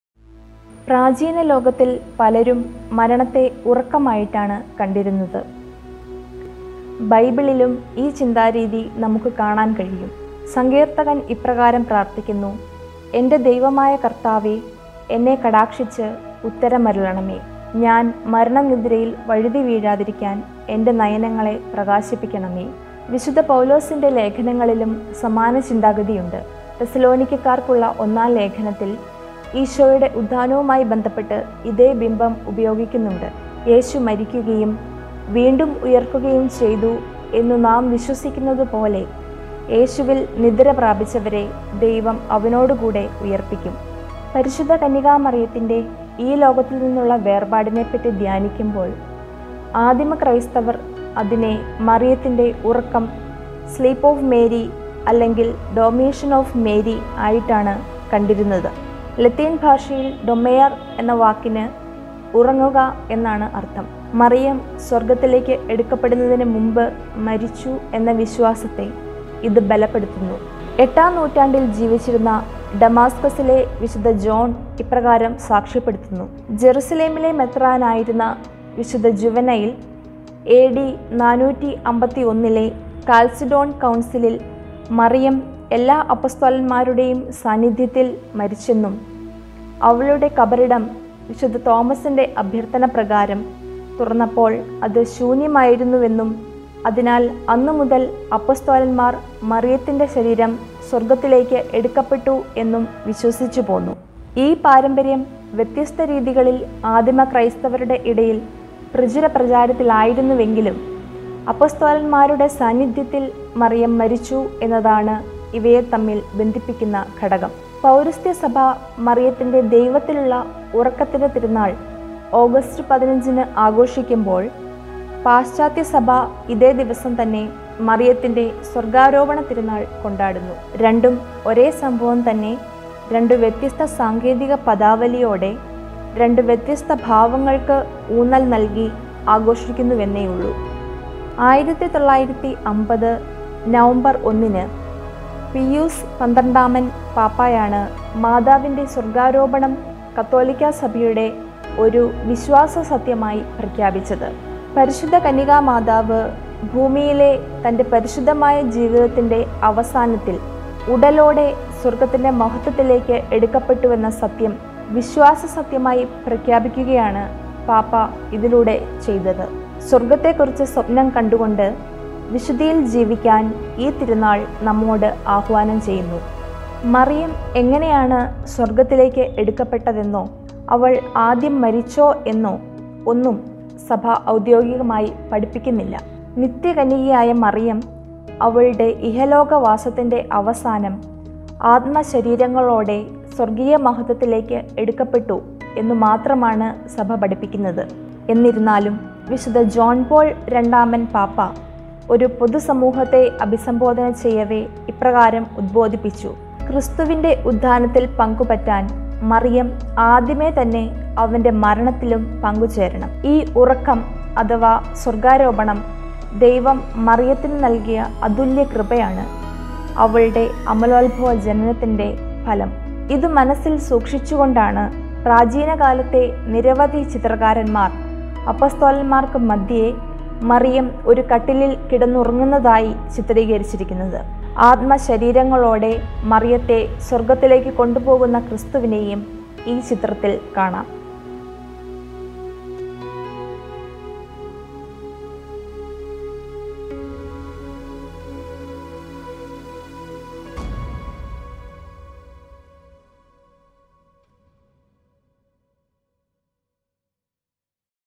പരി. മാതാവിന്റെ സ്വര്‍ഗ്ഗാരോപണം | Assumption of hail Mary
പരി. മാതാവിന്റെ സ്വര്‍ഗ്ഗാരോപണം

1950 നവംബര്‍ 1-ന് പീയൂസ് പന്ത്രണ്ടാമന്‍ പാപ്പായാണ് 'മാതാവിന്റെ സ്വര്‍ഗ്ഗാരോപണം' കത്തോലിക്കാ സഭയുടെ ഒരു വിശ്വാസ സത്യമായി പ്രഖ്യാപിച്ചത്. പരിശുദ്ധ കന്യകാമാതാവ് ഭൂമിയിലെ തന്റെ പരിശുദ്ധമായ ജീവിതത്തിന്റെ അവസാനത്തില്‍, "ഉടലോടെ സ്വര്‍ഗ്ഗത്തിന്റെ മഹത്വത്തിലേക്ക് എടുക്കപ്പെട്ടുവെന്ന" സത്യം വിശ്വാസസത്യമായി പ്രഖ്യാപിക്കുകയാണ് പാപ്പാ ഇതിലൂടെ ചെയ്തത്. സ്വർഗത്തെകുറിച്ച് സ്വപ്നം കണ്ടുകൊണ്ട്, വിശുദ്ധിയിൽ ജീവിക്കാൻ ഈ തിരുനാൾ നമ്മോടു ആഹ്വാനം ചെയ്യുന്നു.